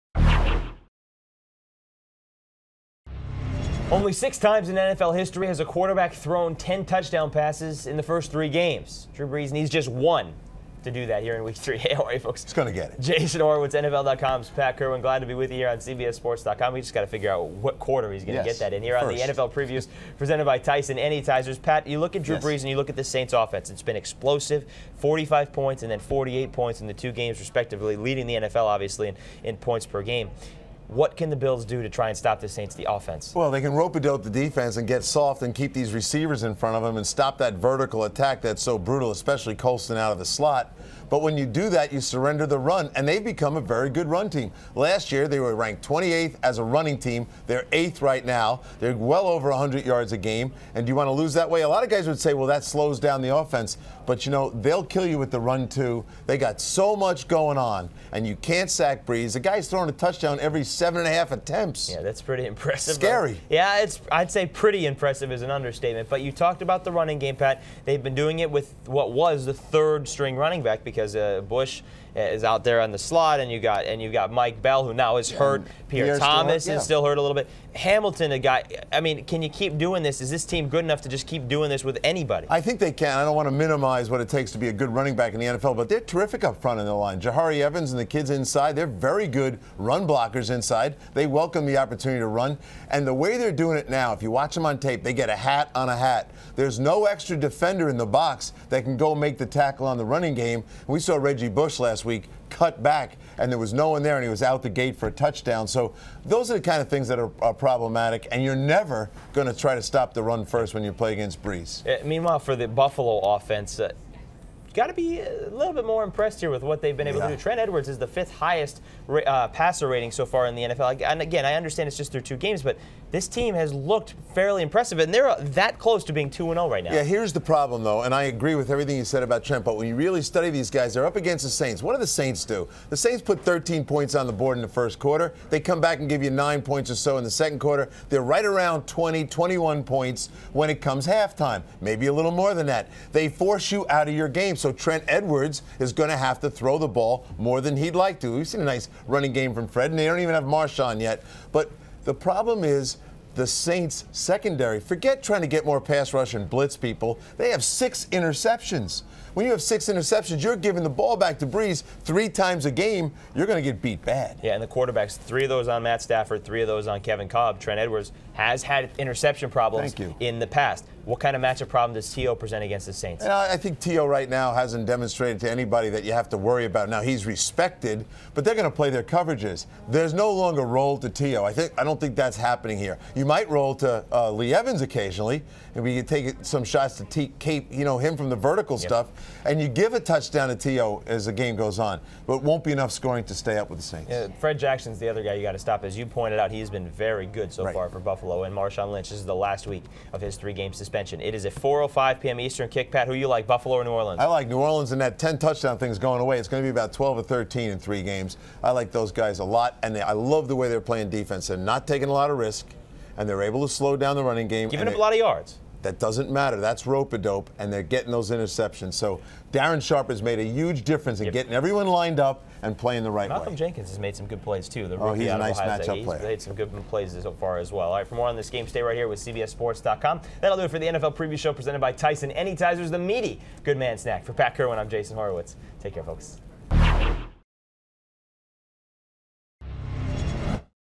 Saints vs. Bills Preview
New Orleans quarterback Drew Brees has been on a tear this season. NFL.com analyst Pat Kirwan and Jason Horowitz see if the Saints signal caller can lead his team to a win over the Bills.